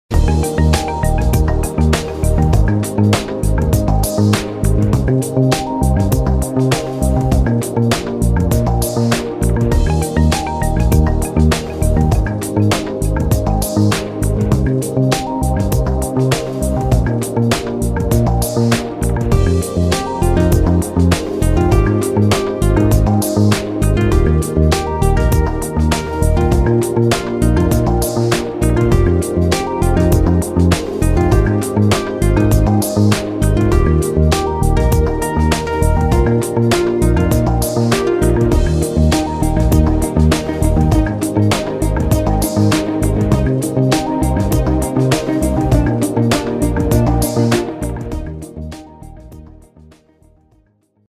Kate Spade New York Ladies' Metro Watch (KSW1142)
Product demonstration Kate Spade New York Ladies' Metro Watch (KSW1142).